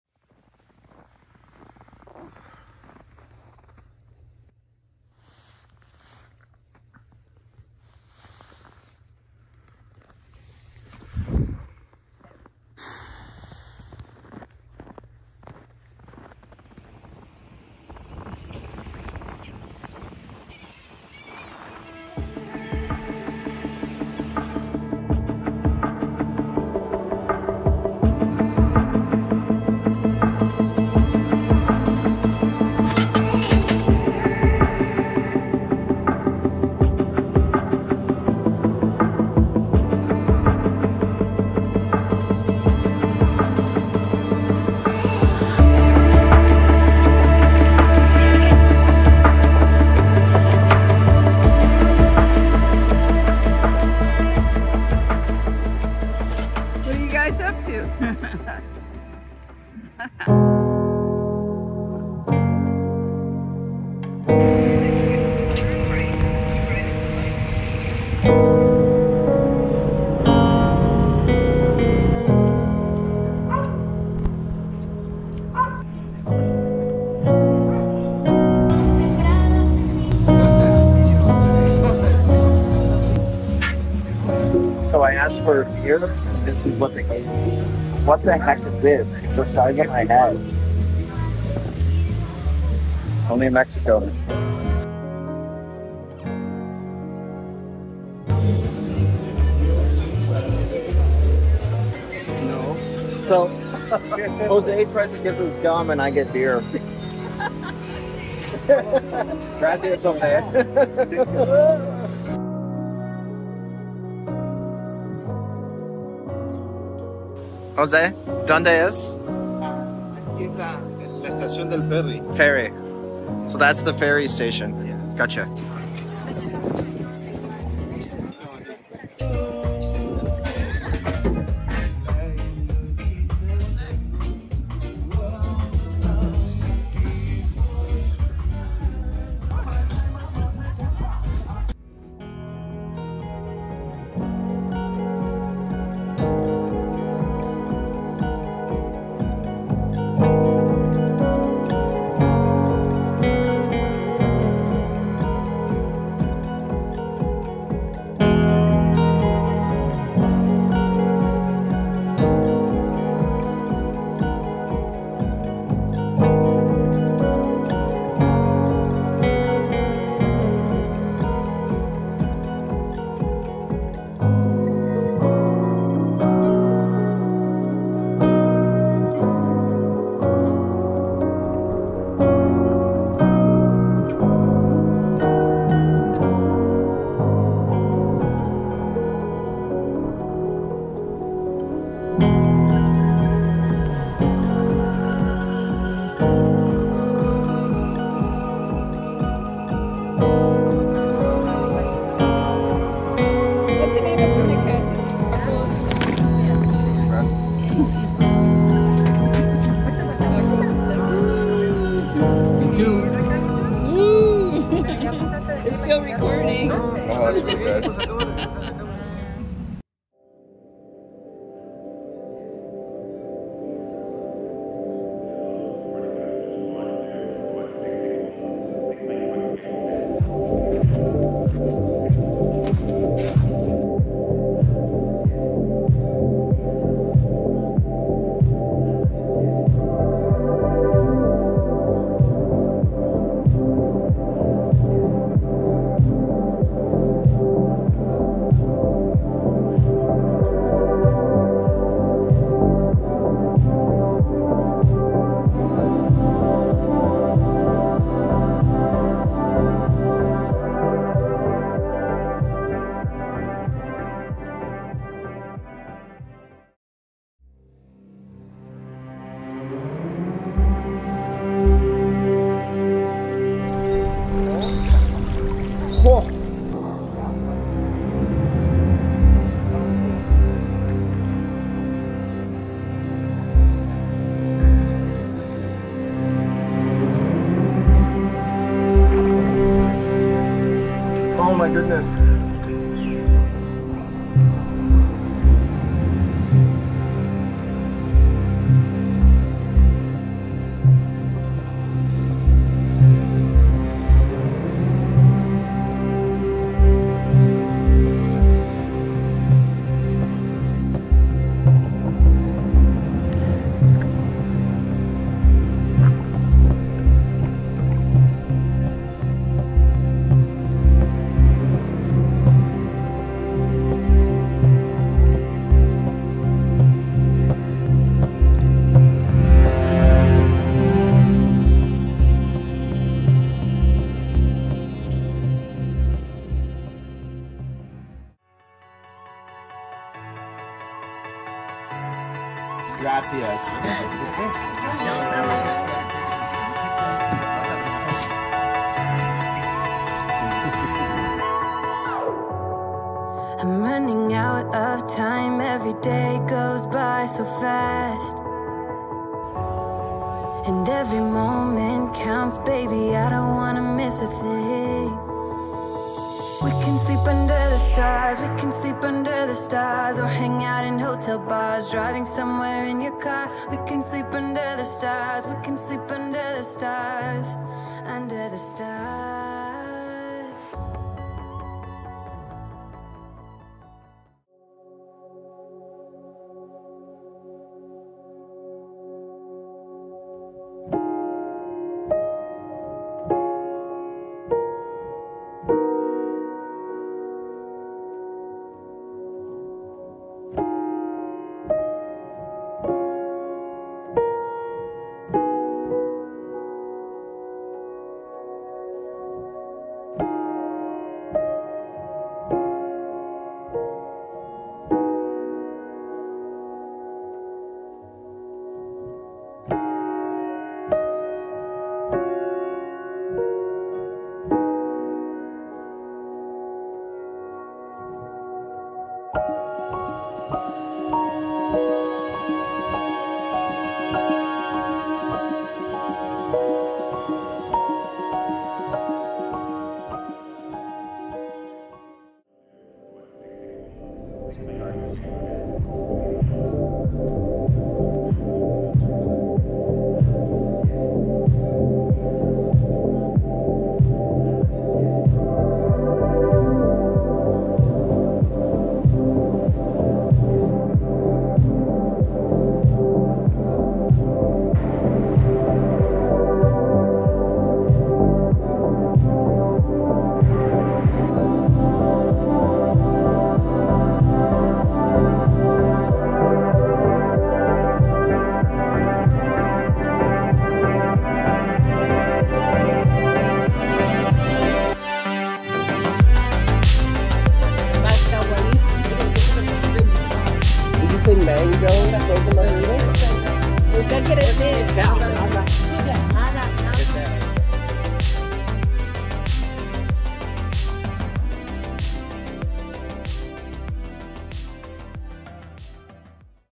Playa del Carmen, My Mini Adventure
My adventure in Playa del Carmen Mexico, I wish I had more time. My week in less then 10 mins.